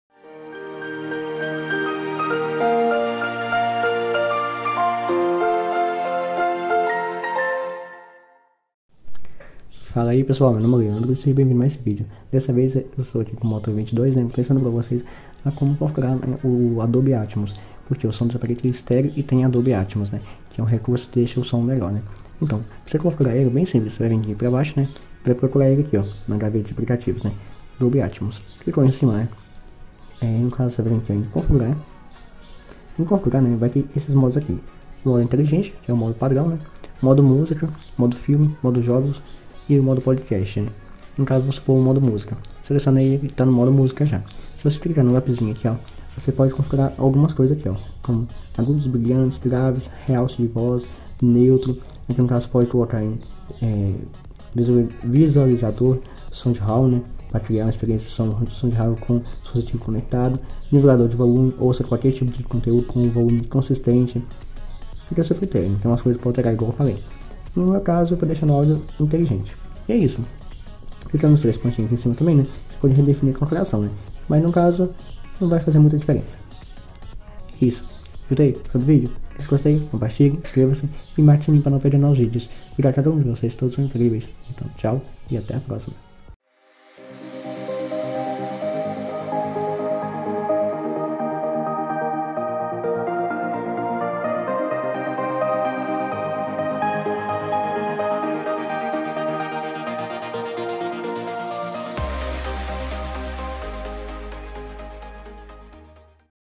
DOLBY ATMOS - MOTO E22
Vou mostrar nesse vídeo COMO CONFIGURAR O SOM DOLBY ATMOS no MOTOROLA MOTO E22